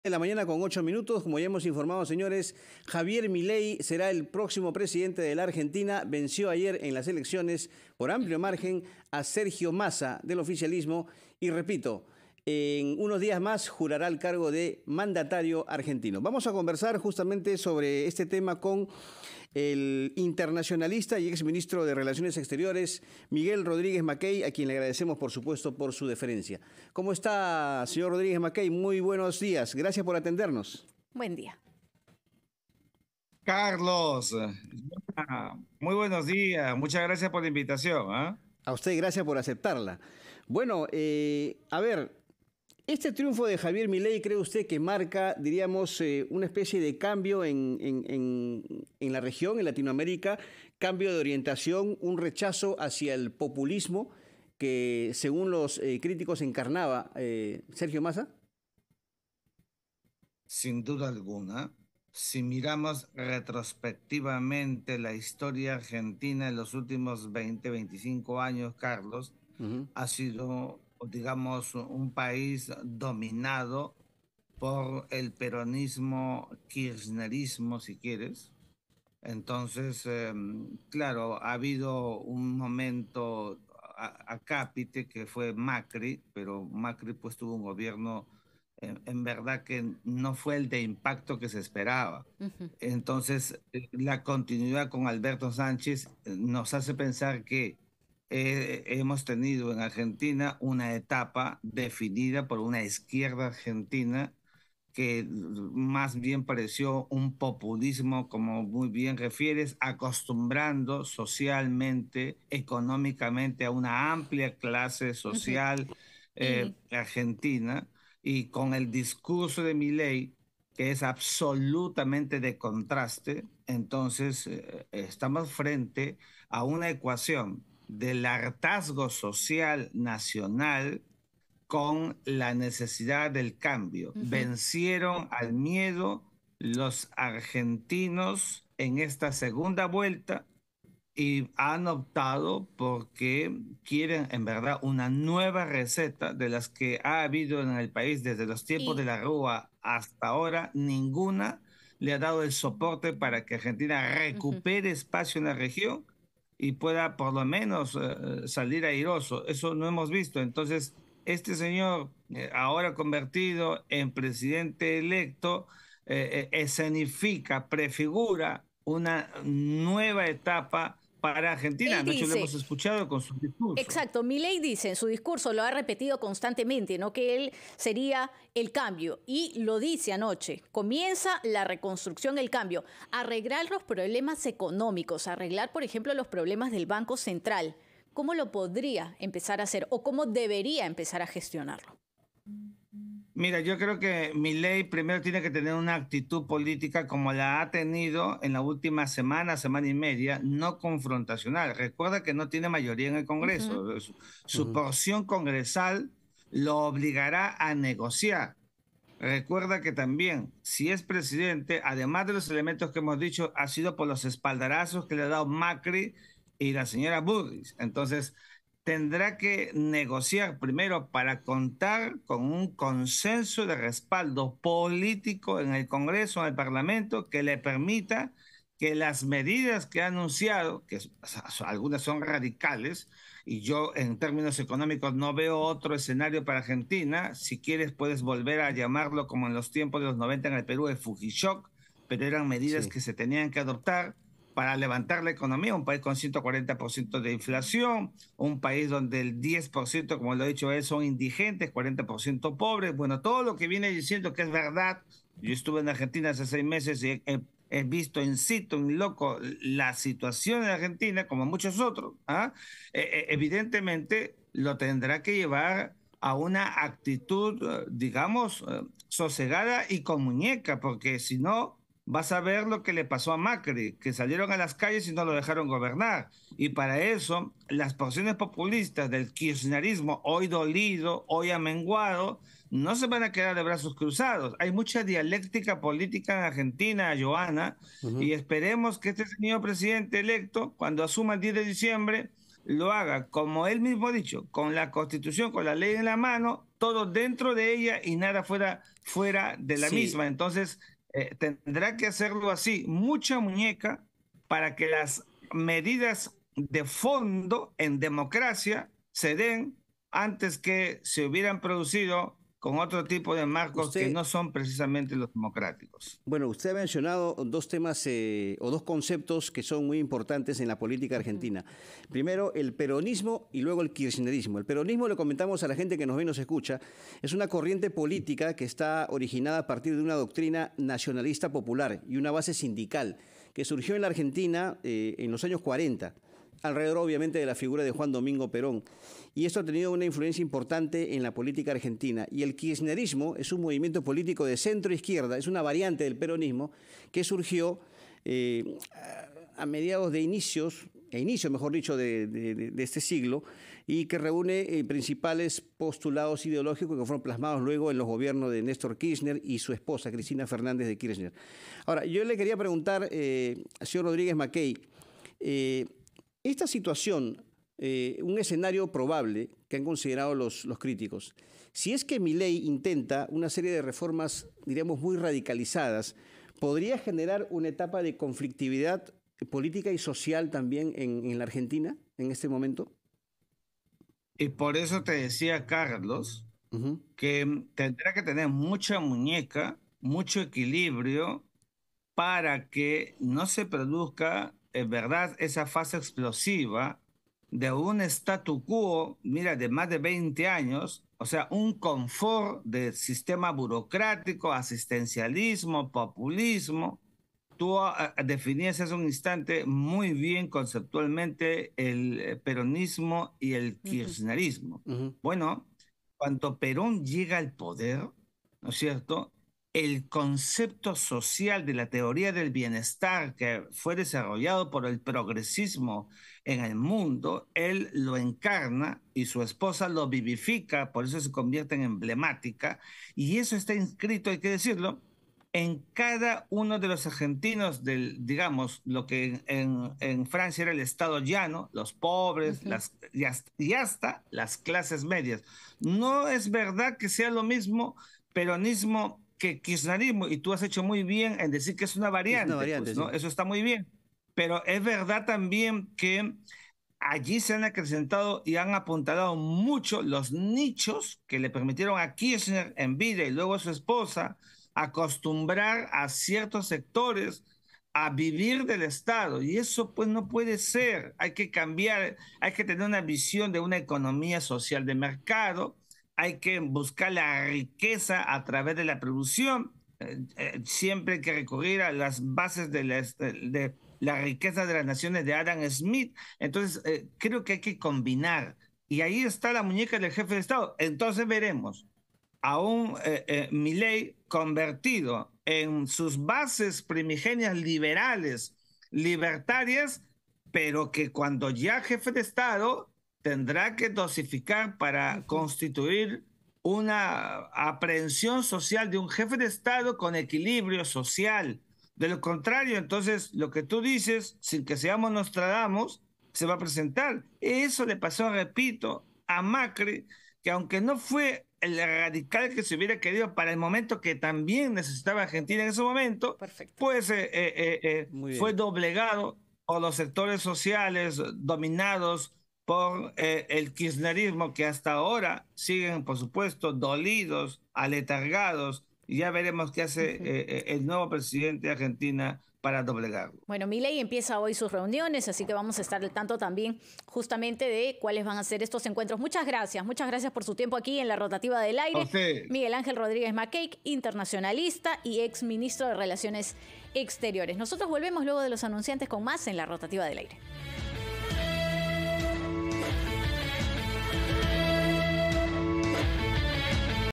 ¿Triunfo de Milei marca un cambio de orientación en Latinoamérica? | Elecciones en Argentina
¿El triunfo de Milei marca un cambio de orientación en Latinoamérica? El internacionalista Miguel Ángel Rodríguez Mackay responde y analiza el resultado de las elecciones presidenciales en Argentina y cuál es el futuro de la región.

rppnoticias RPPNoticias. La voz de todo el Perú. Noticias del Perú y el mundo, las 24 horas del día.